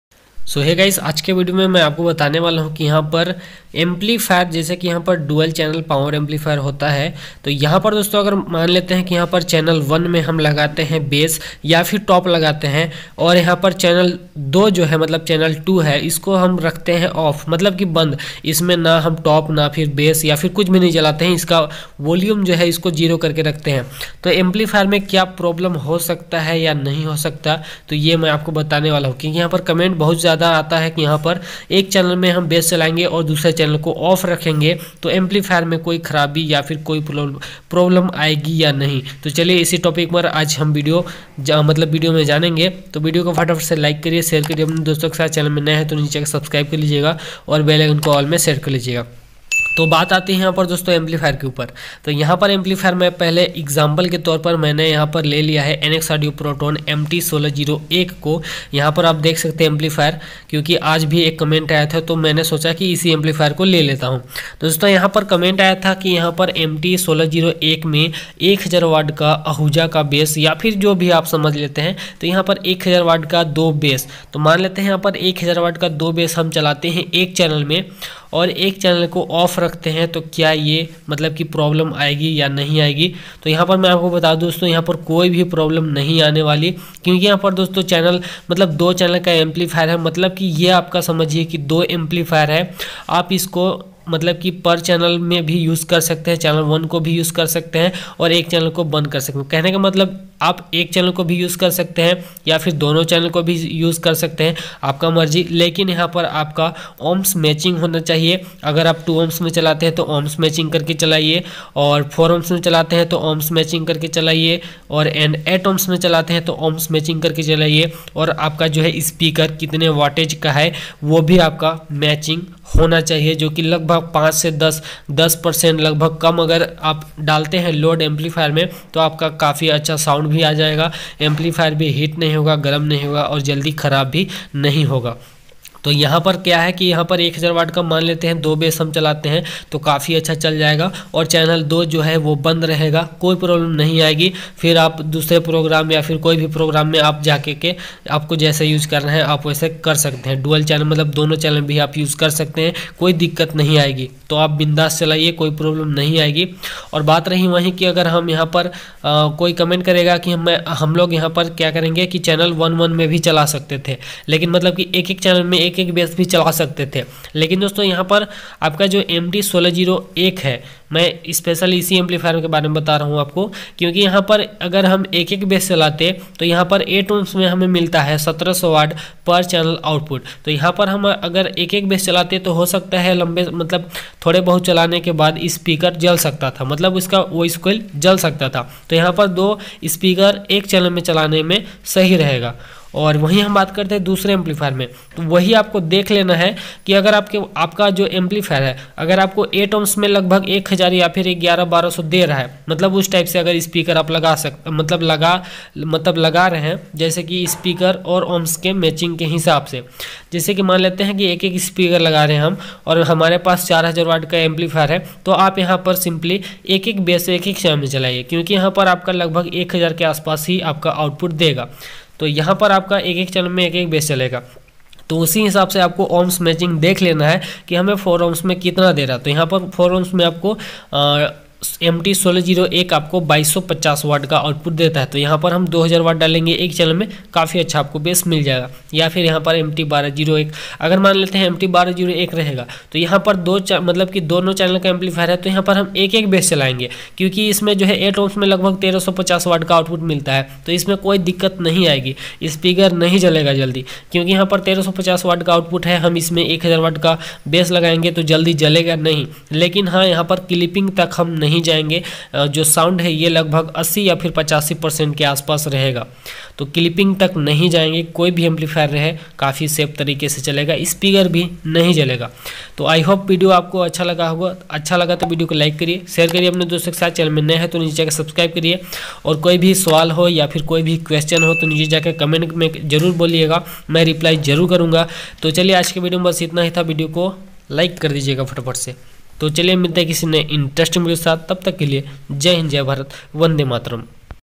1 चैनल चालू और 1 चैनल बन्द तो एम्पलीफायर में कोई दिकत होगी क्या ..?😲 Mt1601
koi bhi amp. me 1chanel chalu or dusra band to kya amplifier kharab ho jayga ya nahi...!

. comments